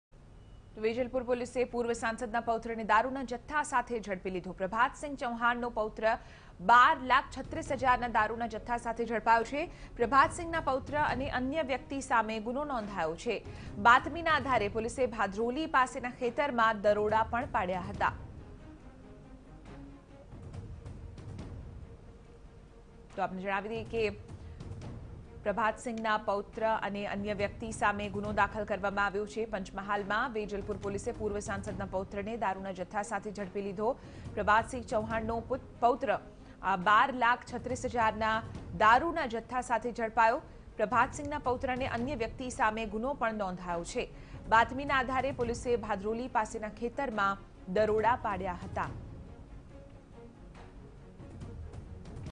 Panchmahal: Grandson of ex-MP Prabhatsinh Chauhan arrested with liquor worth Rs 12.36 lakh | TV9News
Panchmahal: Grandson of ex-MP Prabhatsinh Chauhan arrested with liquor worth Rs 12.36 lakh | TV9News | D3